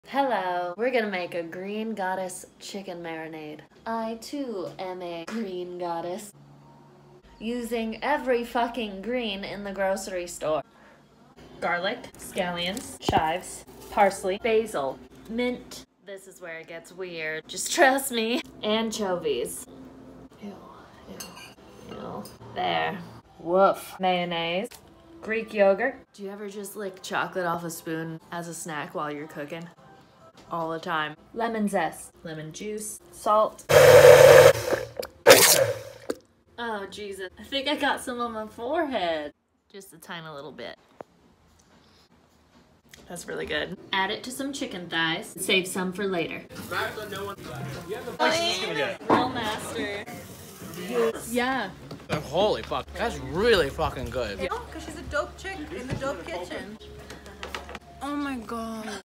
A “Green” Goddess Makes a Green Goddess Marinade
GREEN GODDESS MARINADE💚. This stuff is that good-good. Add it to your favorite cut of chicken and marinate for up to 24 hours or use it as a dressing. Fully customizable recipe below.

3-4 cloves garlic, minced
4 scallions
1/4 cup fresh chives
1/2 cup fresh parsley
20 basil leaves
2 tbsp fresh mint
4 anchovy filets
1/3 cup mayonnaise (I actually used Mexican Crema because it’s all I had)
3/4 cup Greek yogurt

Set some of the marinade aside to use as a dressing. Pour the rest over some boneless skinless chicken thighs (you can use breast too but I hate chicken breast so no thanks). Marinate your meat for at least 4 hours, or up to 24. I made some chicken tacos with my grilled thighs and topped the tacos with more of the dressing, cilantro, jalapeño, and red onion.

I also had so many leftover herbs, so I made this again using lime juice instead of lemon, and adding some jalapeño!

How will you customize your Green Goddess?! 💚